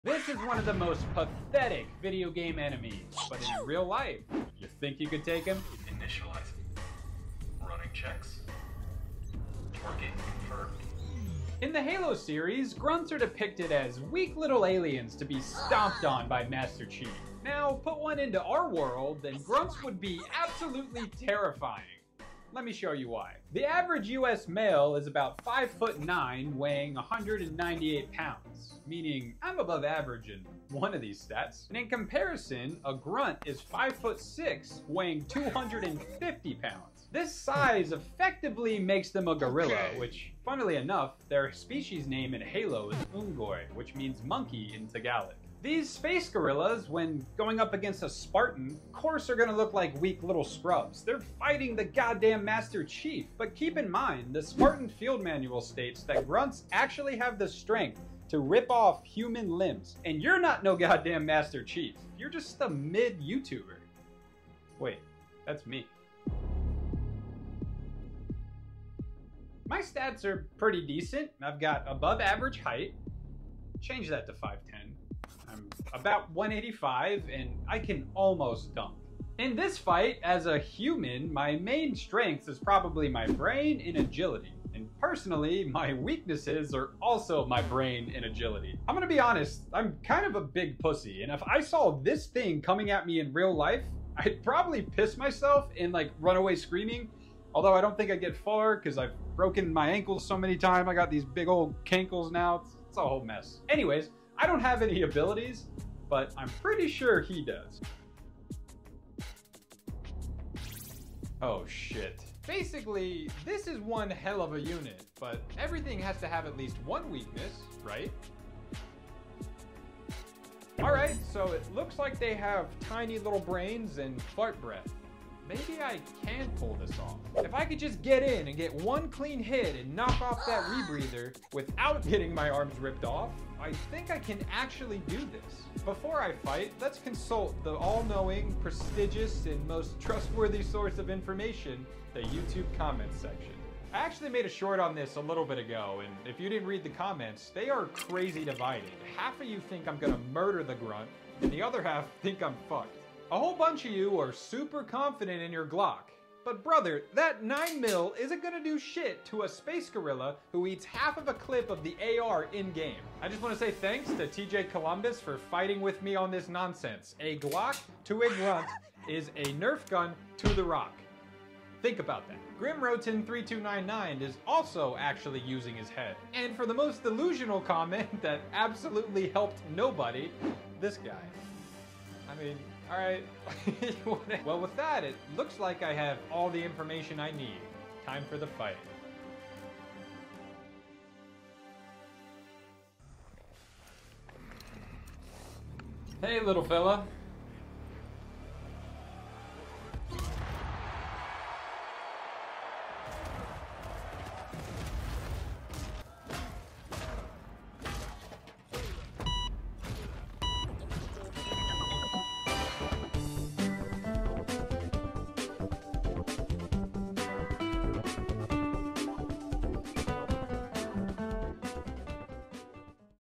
How To Beat A Halo Grunt IRL
In a real life fight, is it possible to beat a grunt? i think i can at least get a few hits in. or maybe not.

I stream Wednesdays on YT @SkaloTV